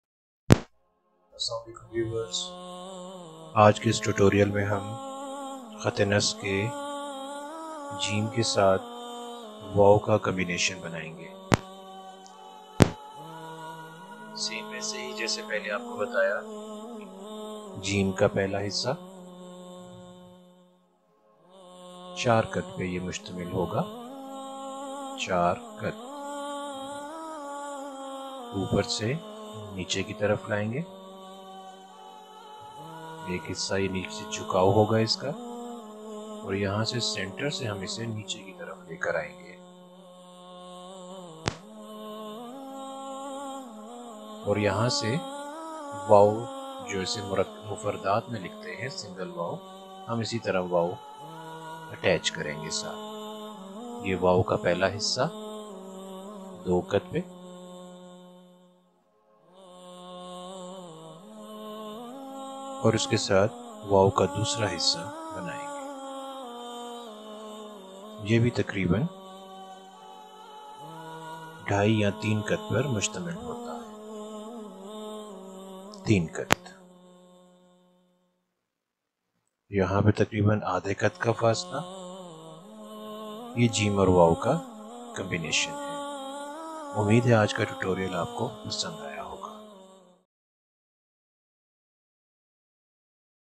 naskh script جو {Lesson 34}
Hi Everyone"
This video belongs to composition of lettering in Arabic format however
In This Tutorial we will discussed that how to write basic Calligraphy techniques of Naskh Script with chalk on black canvas, wherein the learning
concept of Islamic Calligraphy will be batter and some practice work you can be professional that is Practice Makes Perfect.

Thanking you
Artist
MM

calligraphy tutorial
handwriting for beginner
naskh script
art tutorial
step by step
how to learn painting
learn art